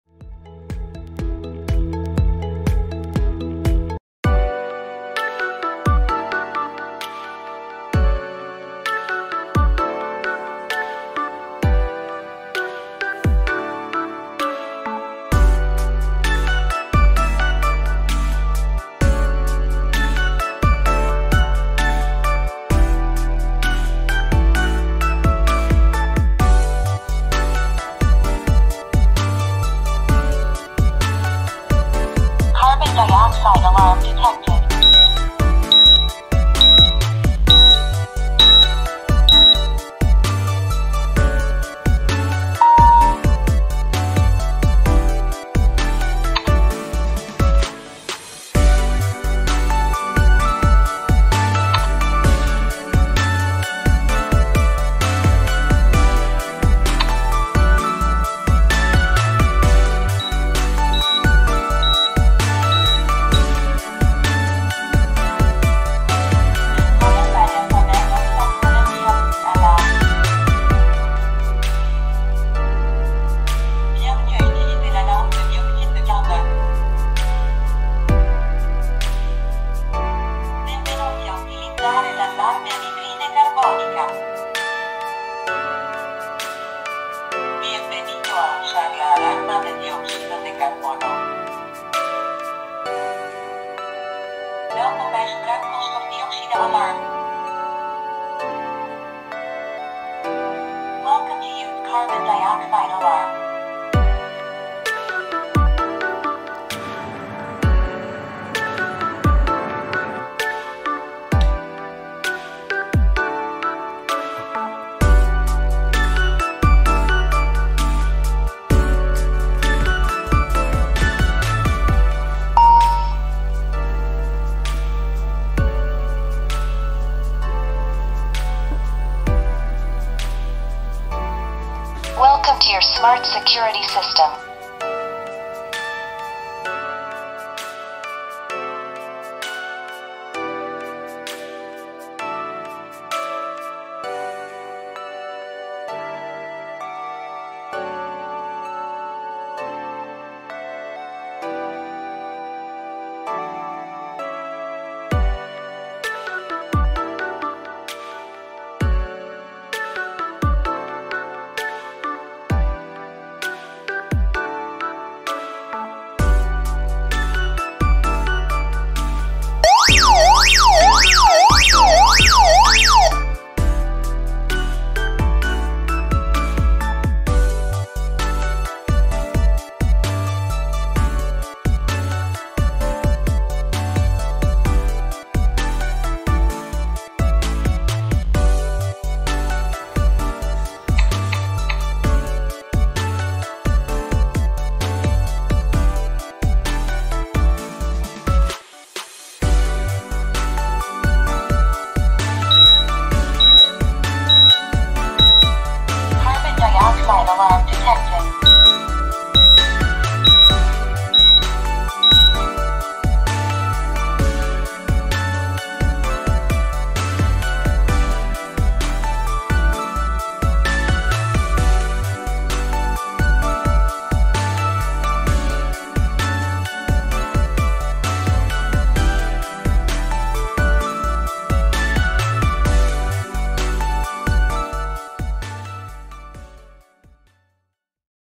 Siterwell GS241A---Wi-Fi Composite carbon dioxide, temperature, and humidity detector
🔹Own display screen, easy to view
🔹With touch button, easy to operate
🔹The backup battery life is more than 12 hours
🔺Can be placed on the desktop or mounted on the wall
🔺The service life of the product can reach 5 years
🔺SiterLink wireless communication protocol is adopted, which is safe, reliable, and convenient